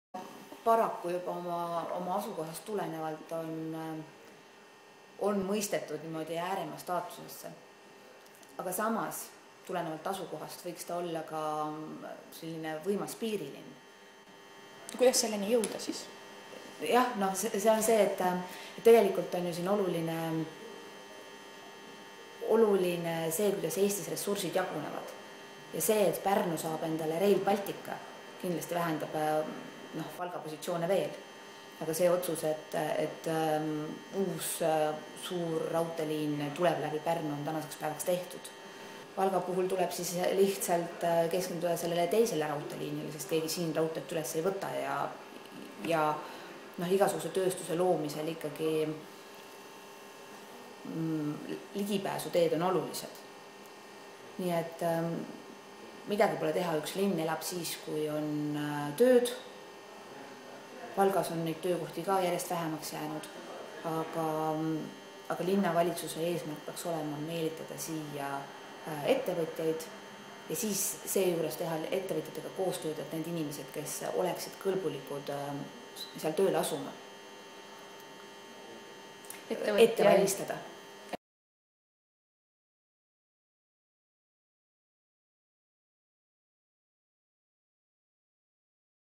Kadri Simson Valga linna rollist täna
Riigikogu liige Kadri Simson sõnastab tänase Valga linna rolli.
Katkend 26.aprillil Valgamaalasele antud intervjuust.